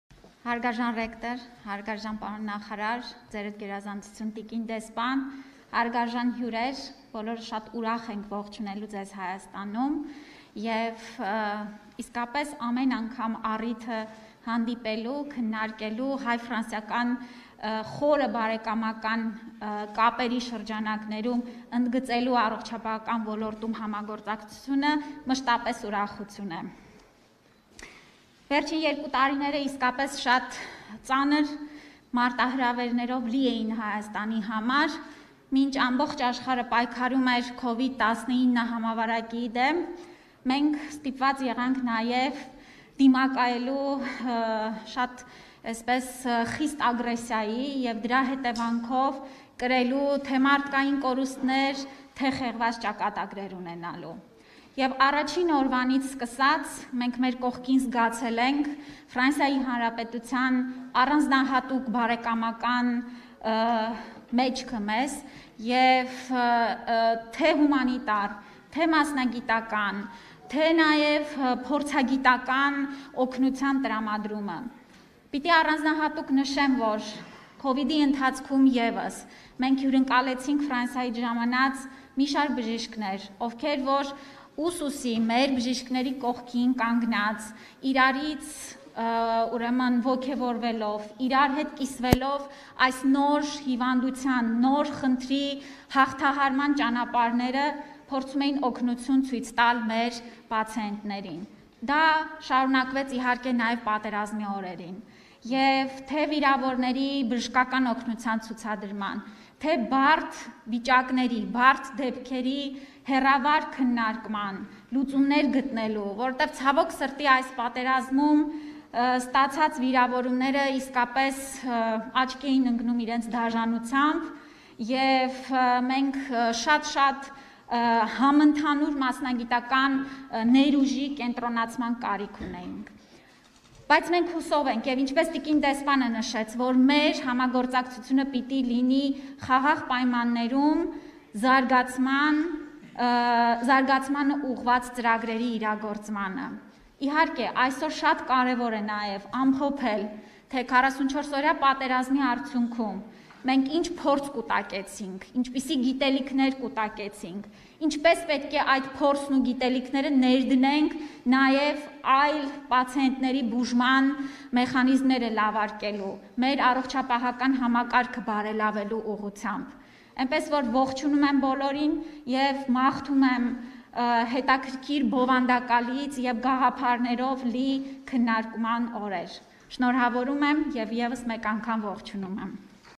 Առաջին օրվանից սկսած զգացել են ֆրանսիացիների առանձնահատուկ բարեկամական մեջքը. Անահիտ Ավանեսյան
ՀՀ առողջապահության նախարար Անահիտ Ավանեսյանը նշեց, որ առաջին օրվանից սկսած զգացել են ֆրանսիացի գործընկերների առանձնահատուկ բարեկամական մեջքը՝ թե՛ հումանիտար, թե՛ մասնագիտական, թե՛ փորձագիտական օգնության տրամադրմամբ:
«Վերջին երկու տարիները, իսկապես, ծանր մարտահրավերներով լի էին Հայաստանի համար: Մինչ ամբողջ աշխարհը պայքարում էր Covid-19 համավարակի դեմ, մենք ստիպված եղանք դիմակայել խիստ ագրեսայի, դրա հետևանքով կրելով մարդկային կորուստներ, և ունենալով խեղված ճակատագրեր»,- ասաց գերատեսչության ղեկավարը՝ կարևորելով առողջապահական համակարգը բարելավելու նպատակով 44-օրյա պատերազմի ընթացքում կուտակած փորձի և գիտելիքների ամփոփումը: